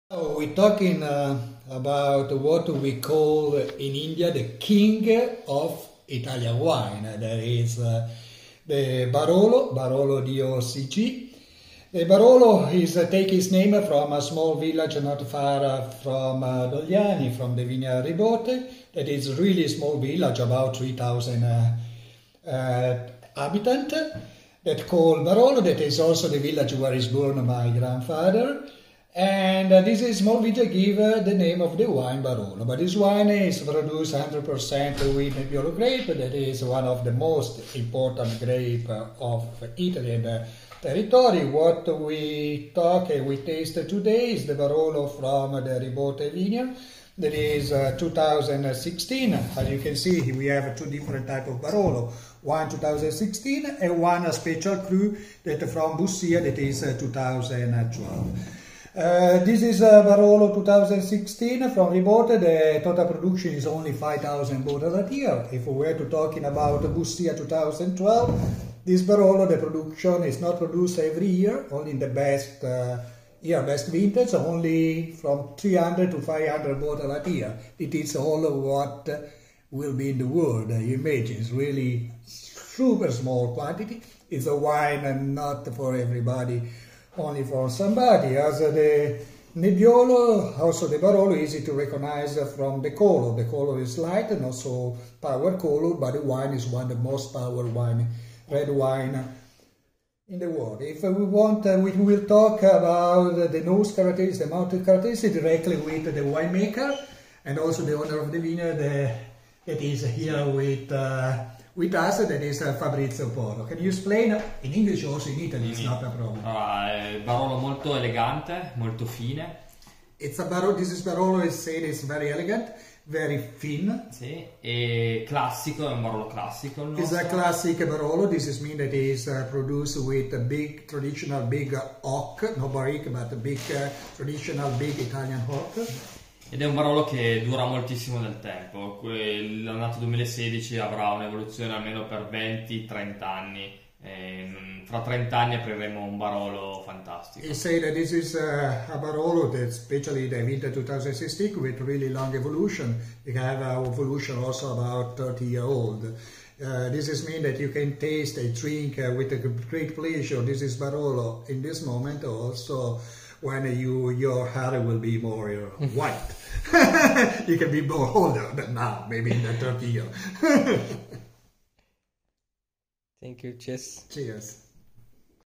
Barolo Docg 2016 and Barolo Docg Bussia by the winery Ribote
Speaking about Barolo Docg produce by the family winery RIBOTE with the international wine consulting Mr. Sebastiano Ramello and the wine maker Fabrizio Porro together with our friends and customer from India.
In India the wine lover like call the Barolo Docg: "The king of italian wines".